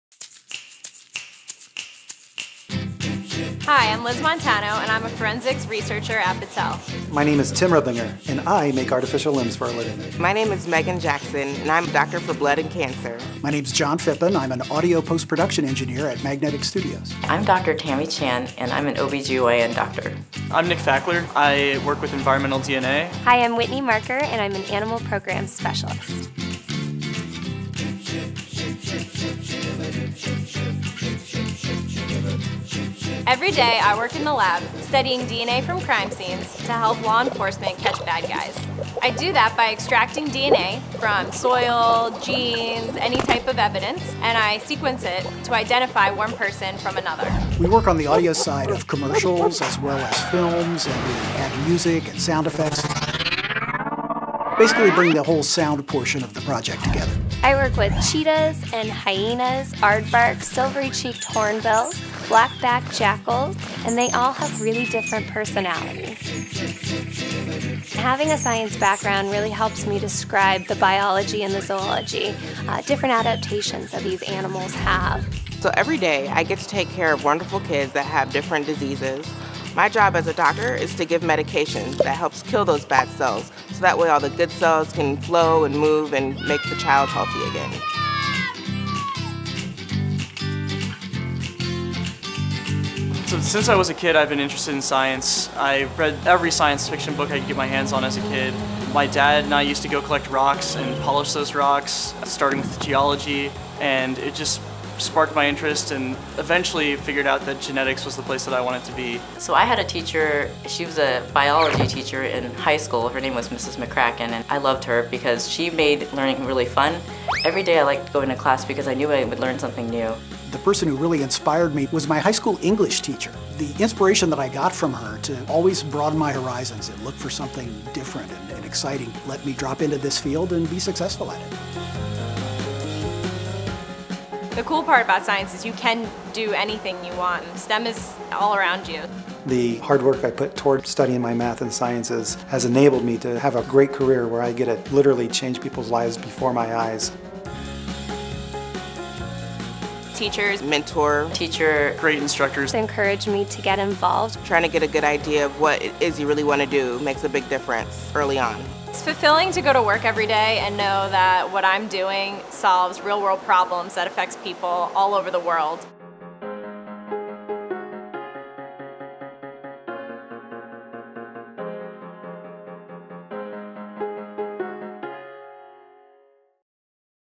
STEM Careers: Inspire the Next Generation of Innovators
STEM careers are fueled by inquiry. In this engaging video, STEM professionals explain where their career paths began and the impact they make through their work.

Learn more about Inspire Science at inspire-science.com.